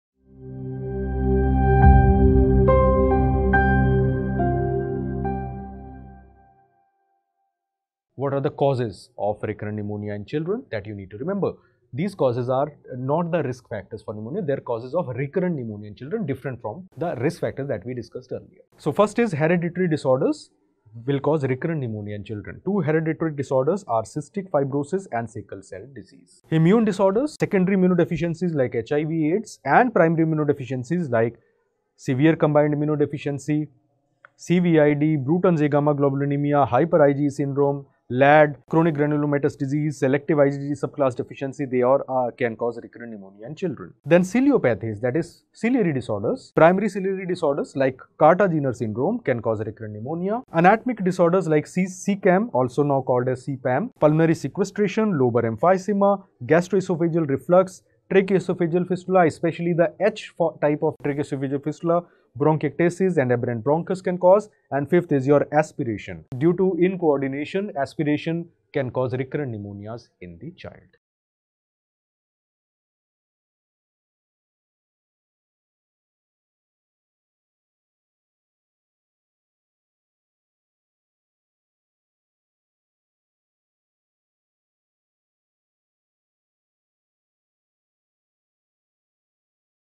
"Causes of Recurrent Pneumonia" by Dr. Sandeep Sharma
Watch Dr. Sandeep Sharma's video on "Causes of Recurrent Pneumonia", a valuable resource for NEET Super Specialty in Pediatrics preparation. Gain insights into managing this condition effectively, equipping yourself with the knowledge needed for success in the exam.